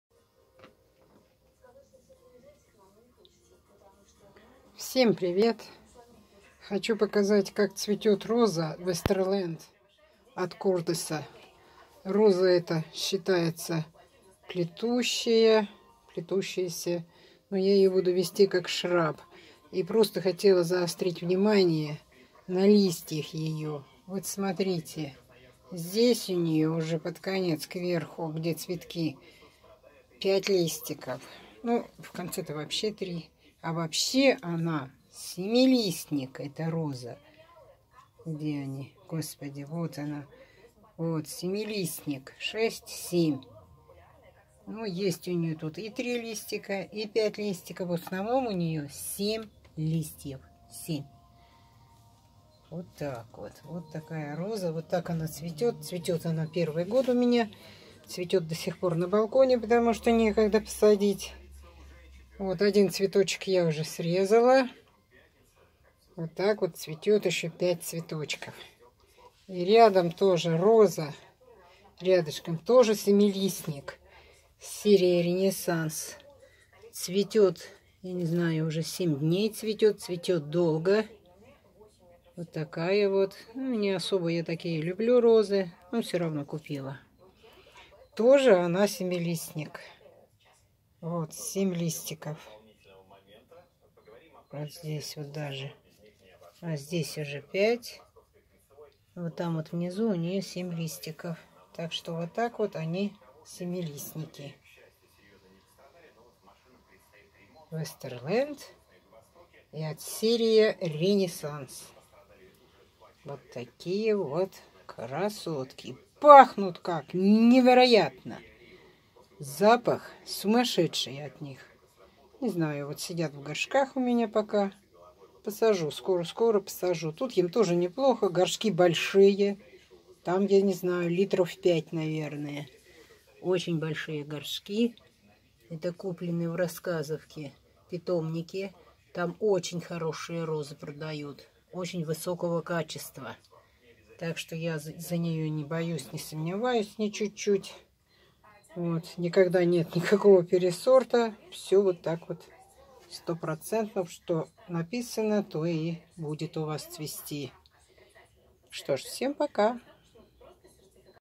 Роза ВЕСТЕРЛЕНД и РЕНЕССАНС серия из питомника в д.РАССКАЗОВКА.
Питомник растений УЛЬЯНОВСКИЙ.Киевское шоссе , Подмосковье.